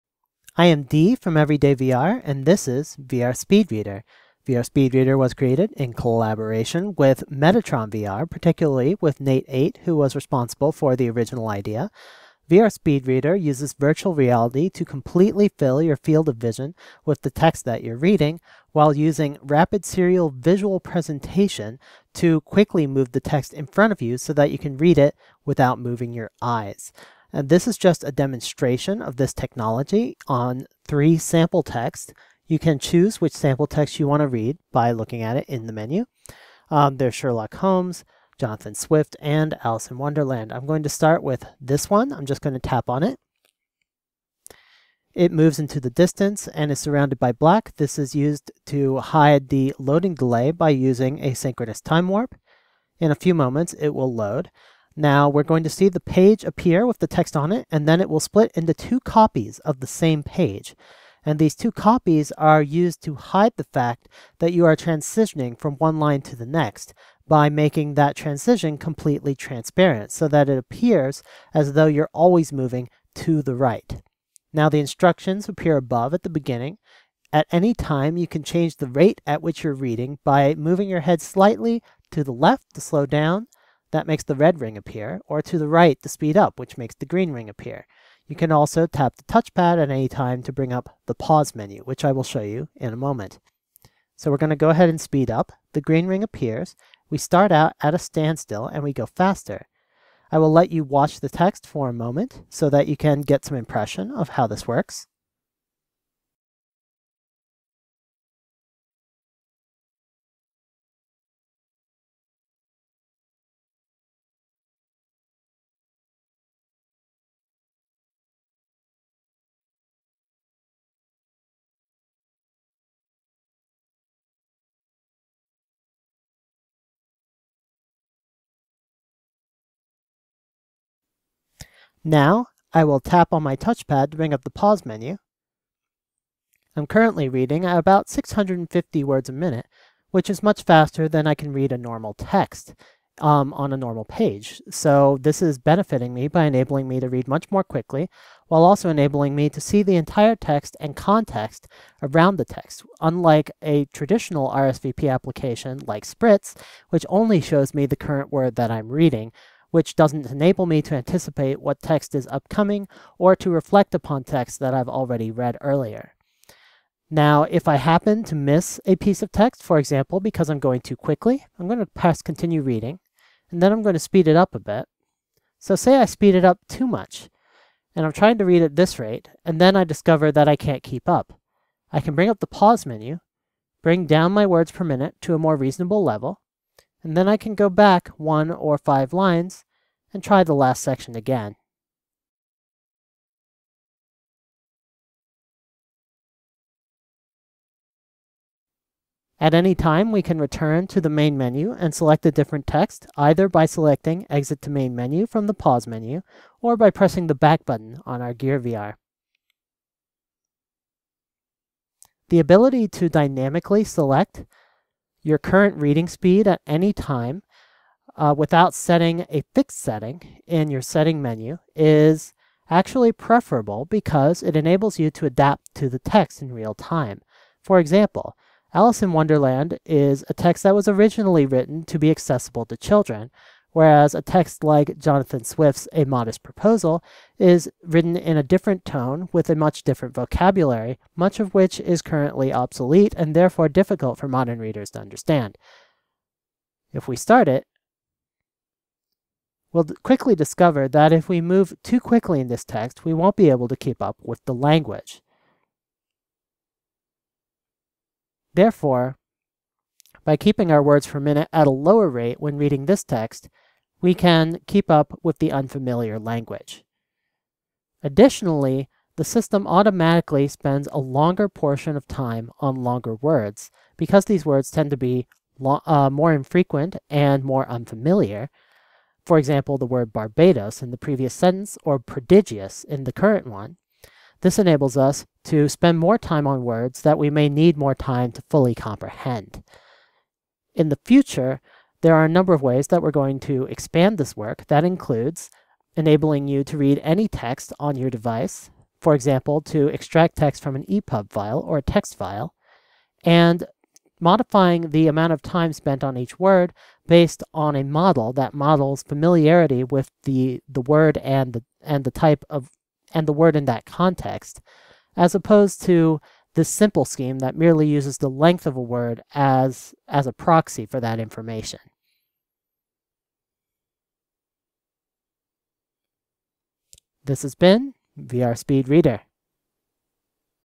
Mobile VR Jam: VR Speed Reader (narrated overview)
I summarize my Mobile VR Jam entry, a simple experimental application for speed reading in virtual reality on Gear VR using Rapid Serial Visual Presentation. It currently has three sample texts. Future work includes reading text from epub and text sources, and improvements to the language model.

If you have a Gear VR, try it yourself!